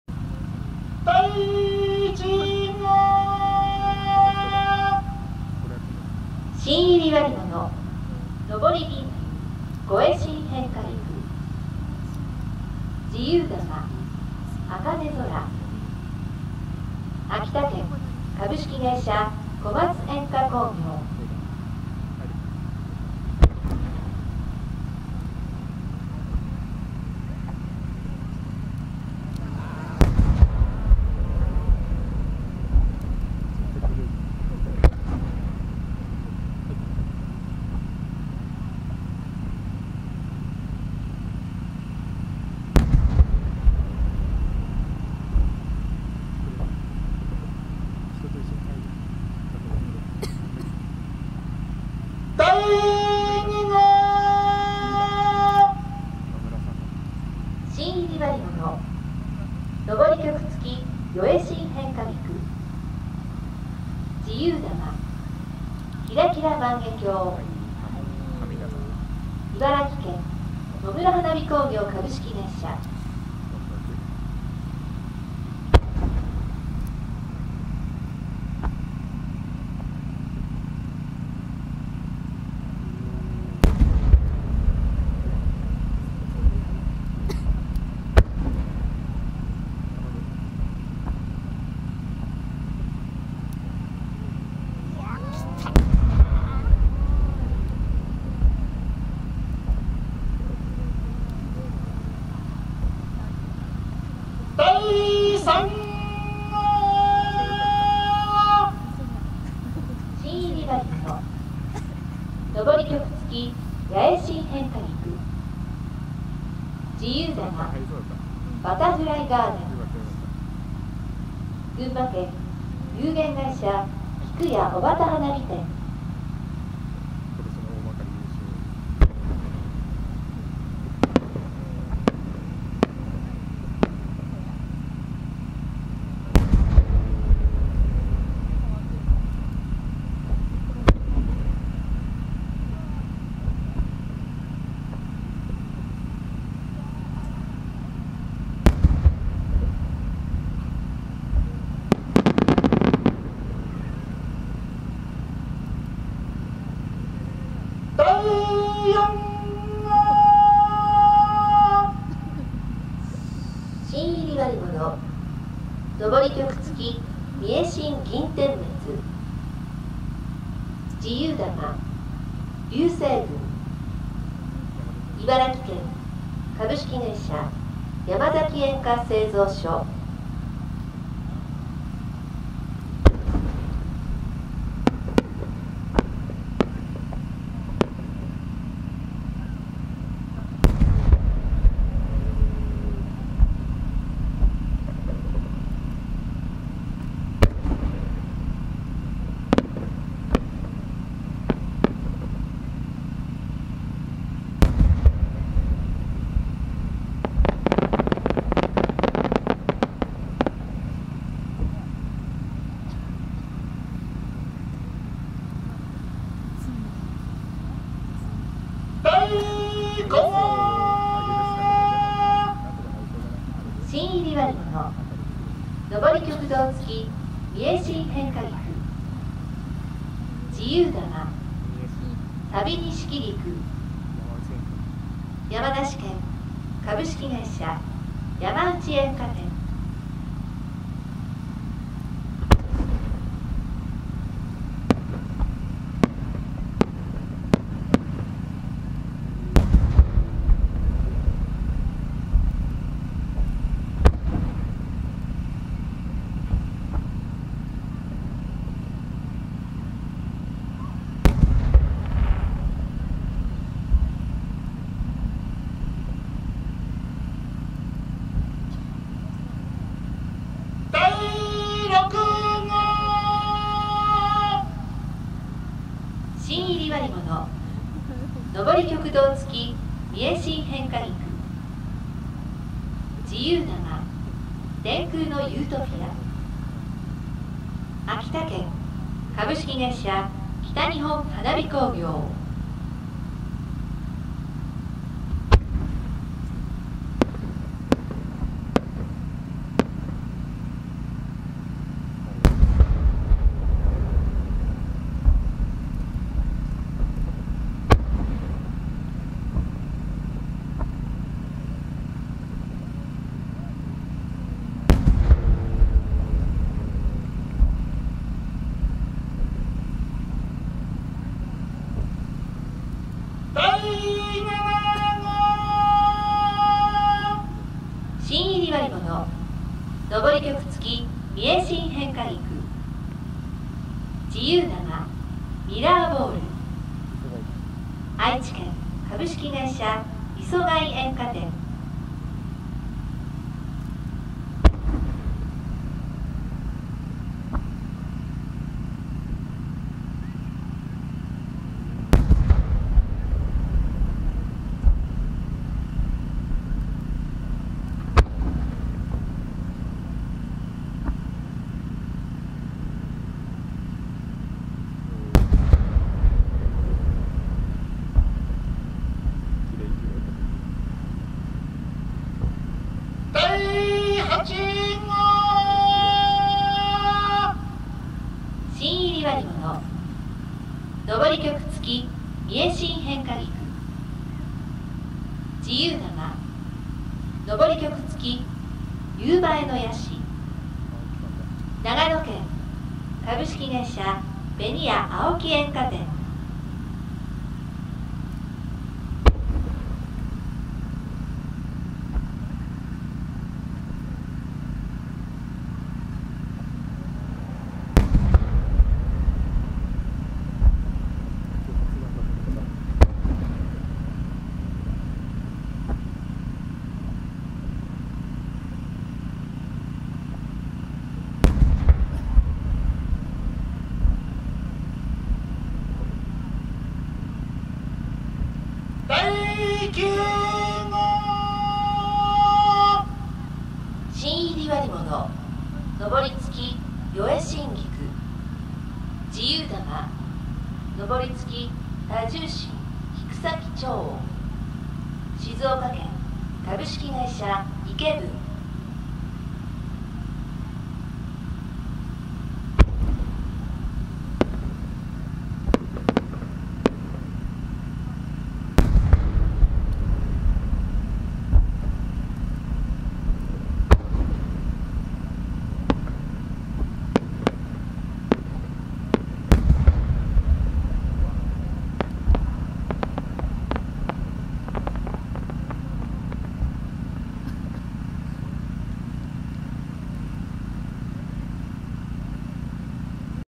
【4K】2018 大曲の花火 秋の章 日本花火作家選抜「伝統割物と斬新自由玉」共演
①は芯入り割物、②は自由玉

１小松煙火工業
①昇銀竜五重芯変化菊　②茜空

２野村花火工業
①昇曲付四重芯変化菊　②キラキラ万華鏡

３菊屋小幡花火店
①昇曲付八重芯変化菊　②バタフライ・ガーデン

４山崎煙火製造所
①昇曲付三重芯変化菊　②流星群

５山内煙火店
①昇曲導付三重芯変化菊　②寂錦菊

６北日本花火興業
①昇曲導付三重芯変化菊　②天空のユートピア

７磯谷煙火店
①昇曲付三重芯変化菊　②ミラーボール

８紅屋青木煙火店
①昇り曲付三重芯変化菊　②昇り曲付夕映えの椰子

９イケブン
①昇り曲四重芯菊　②昇り付多重芯菊先潮音

【大会名】大曲の花火 秋の章
【開催場所】秋田県大仙市大曲雄物川河畔
【撮影場所】カメラマン席
【プログラム】
【担当煙火】